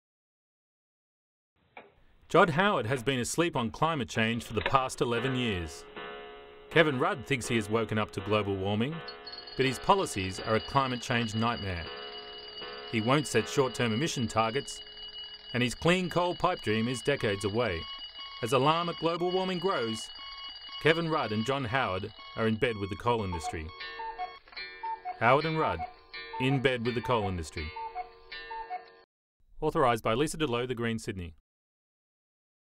Howard and Rudd in bed with the coal industry - Greens Ad
A spoof of Labor's 'John Howard is asleep on climate change' ad.

The ad shows John Howard and Kevin Rudd in bed together and highlights the dangers of Labor and Liberal policies on global warming.

Despite the threat of climate change, Kevin Rudd has refused to set short term emission reduction targets, has committed to an expansion of the coal industry and the continued logging of old growth forests. Unproven "clean coal" - that won't be available for decades -- is favoured by Labor over renewable energy such as wind, solar and geothermal. Rudd and Howard have also both ruled out a carbon tax to reduce emissions.